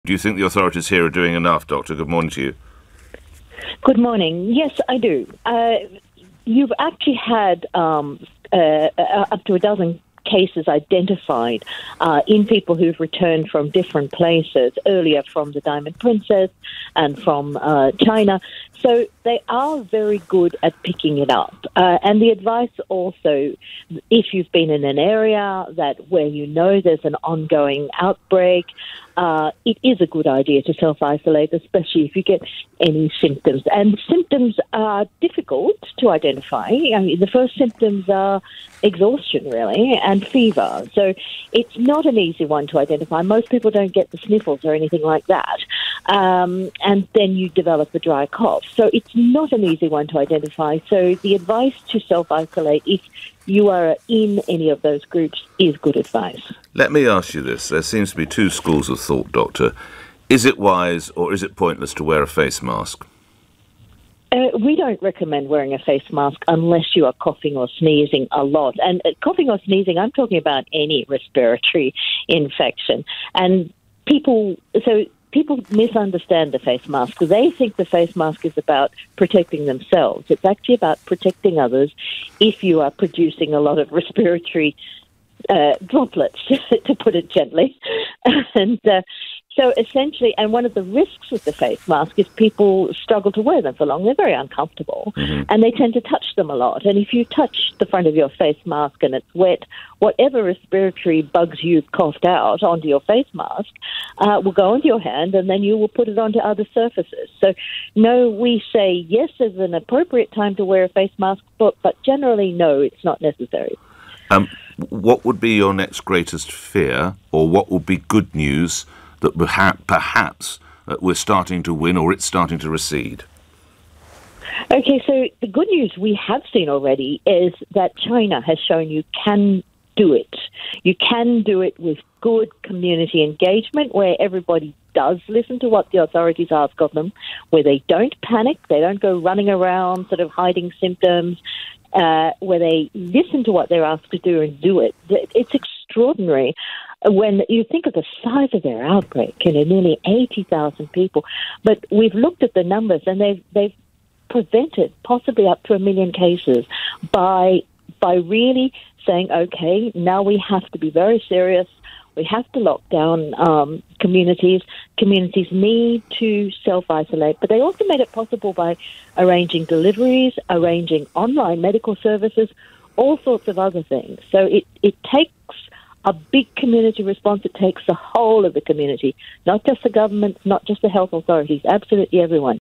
Expert explains why face masks won't protect you from Coronavirus Covid-19
Facemasks will not protect you from catching coronavirus, an expert from the World Health Organisation has told LBC.

Facemasks are becoming more commonplace in both the UK and around Europe after an outbreak of the killer virus in northern Italy.

But speaking to Nick Ferrari, Dr Margaret Harris from the WHO said the masks may protect other people from your germs - but won't protect you.